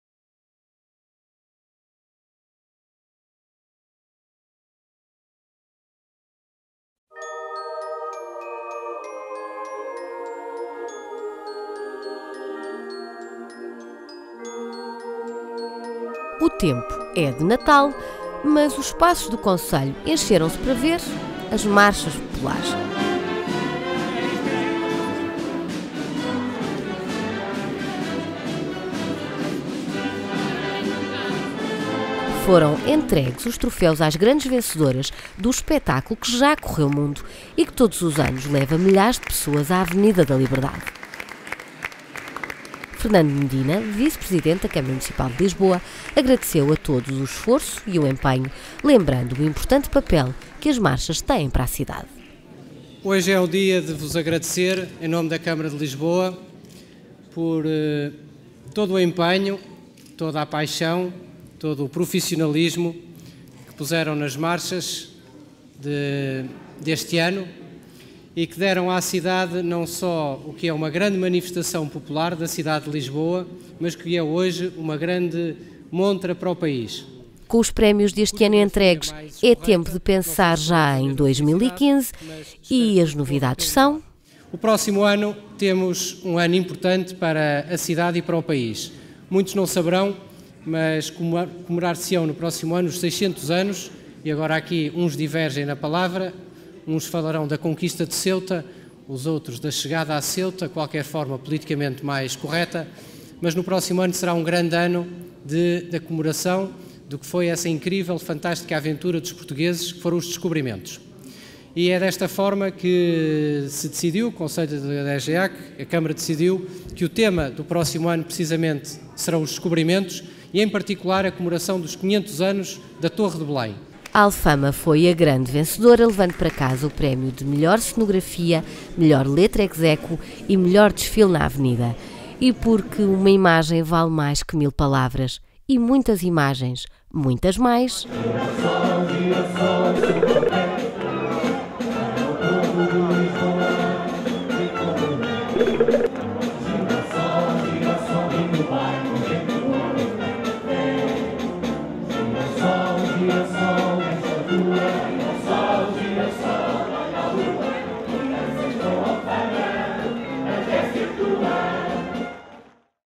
Cerimónia de entrega dos Troféus das Marchas Populares 2014
Teve lugar no passado dia 10 de dezembro, no Salão Nobre dos Paços do Concelho a cerimónia de entrega dos troféus às marchas que este ano se distinguiram nas diferentes categorias pelo júri das marchas e onde compareceram em grande número os representantes das marchas. Um cavalinho (agrupamento musical que participa nas marchas da cidade) fez as honras proporcionando o ambiente musical que se impunha à cerimónia.